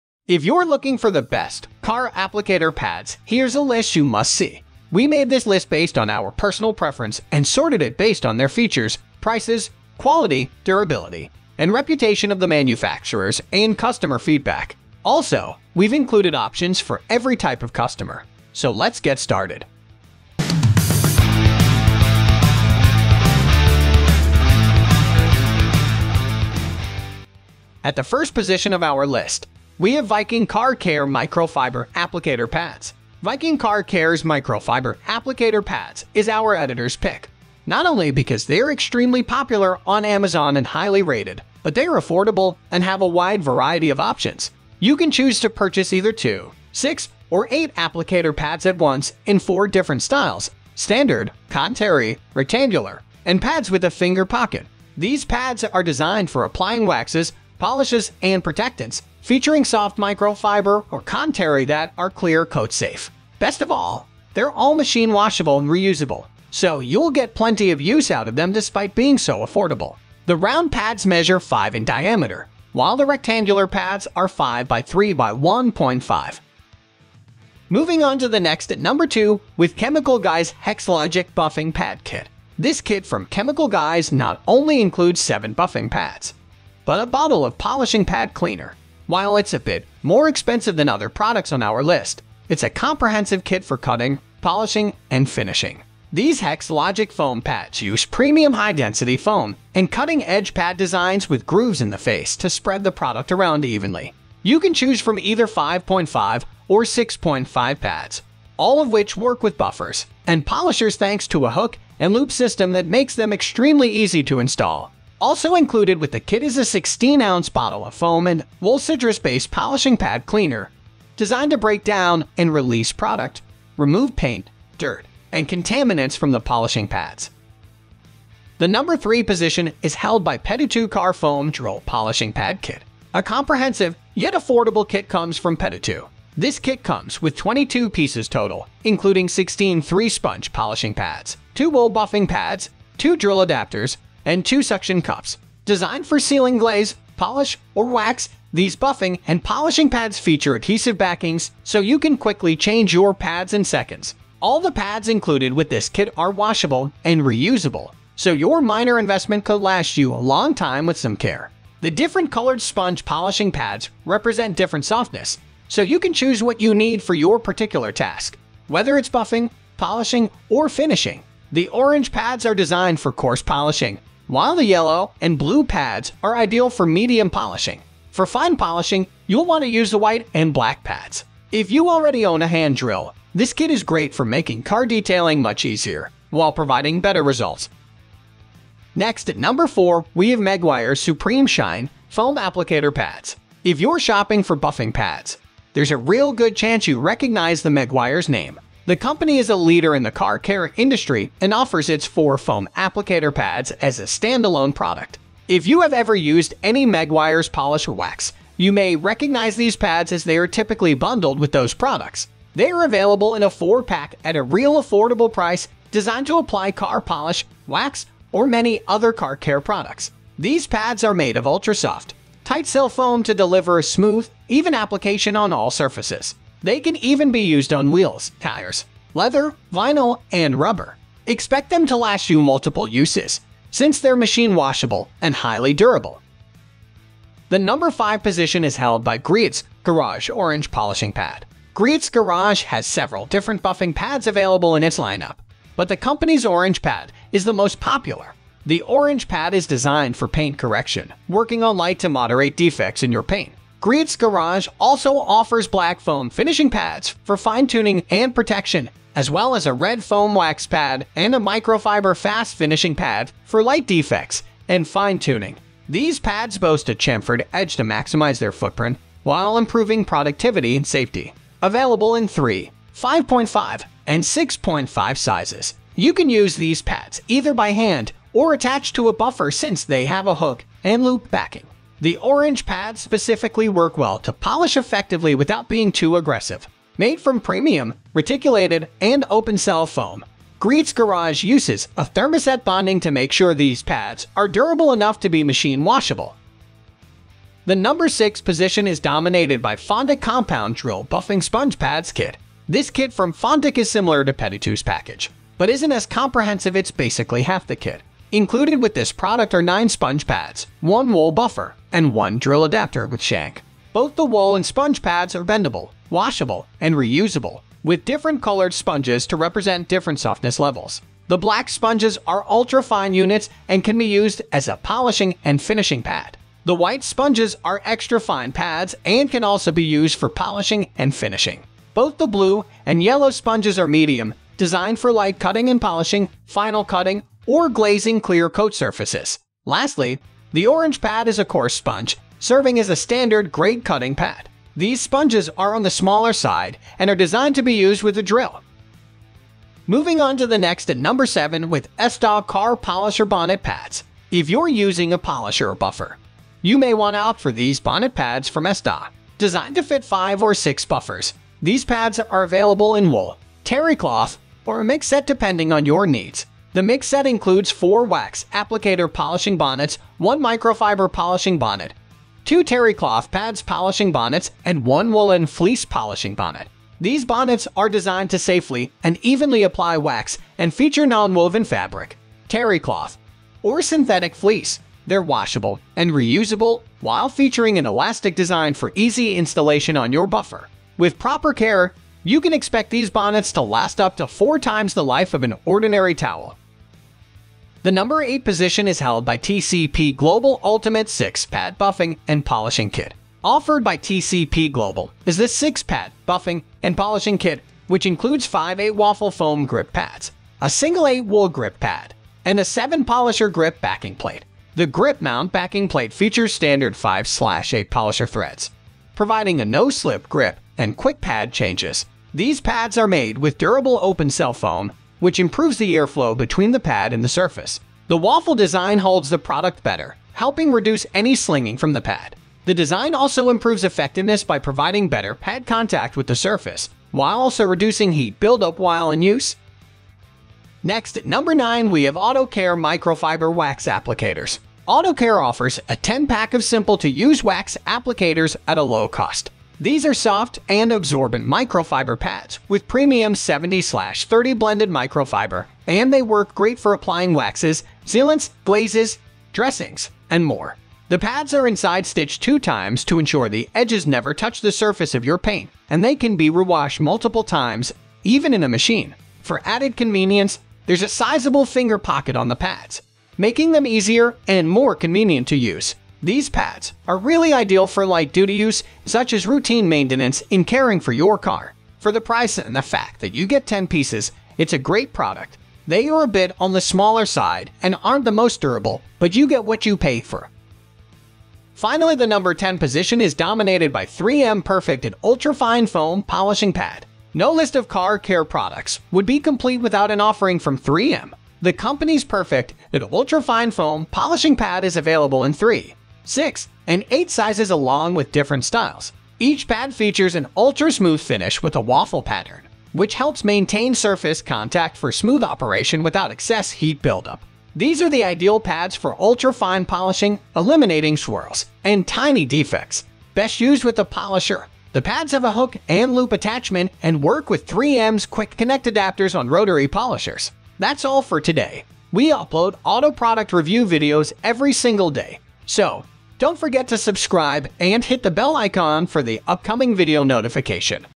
Best Car Applicator Pad In 2024 - Top 10 Car Applicator Pads Review
Links to the best Car Applicator Pads we listed in today's Car Applicator Pads review video:

1 . Viking Car Care Microfiber Applicator Pads

2 . Chemical Guys Hex-Logic Buffing Pad Kit

3 . Petutu Car Foam Drill Polishing Pad Kit

4 .  Meguiar’s Supreme Shine Foam Applicator Pads

5 . Griot's Garage Orange Polishing Pad

6 . Fontic Compound Drill Buffing Sponge Pads Kit

7 . SPTA Car Polisher Bonnet Pads

8 . TCP Global Ultimate 6 Pad Buffing and Polishing Kit

9 . AutoCare Microfiber Wax Applicators

10 . 3M Perfect-It Ultrafine Foam Polishing Pad

What are the Best Car Applicator Pads in 2024?
In today's video we reviewed the top 10 best Car Applicator Pads on the market in 2024. We made this list based on our personal opinion and we ranked them in no particular order, after doing our research based on their prices, quality, durability, brand reputation and many more.

After watching this video you will know which are the best budget, top selling and top rated Car Applicator Pads on the market today.

If you choose from this list you can be sure that you buy one of the best Car Applicator Pads available today.